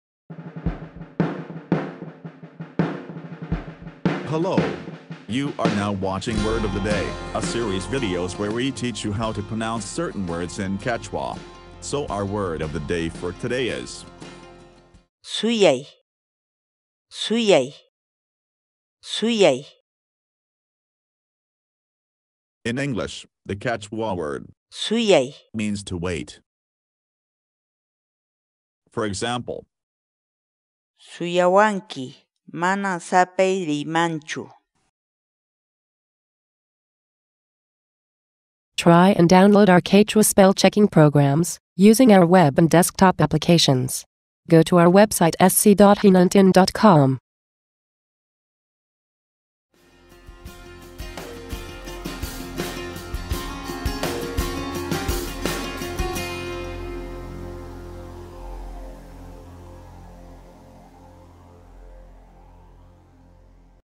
How to pronounce suyay ‘to wait’
Watch the video now on
Social

Listen to our podcasts on SoundCloud  ► hinant.in/uyarinaypaq
Follow us on Google Plus  ► hinant.in/googleplusnisqapi

Hinantin Research Group: The entry ‘suyay‘ is part of the basis for the development of a freely available talking dictionary for Quechua language.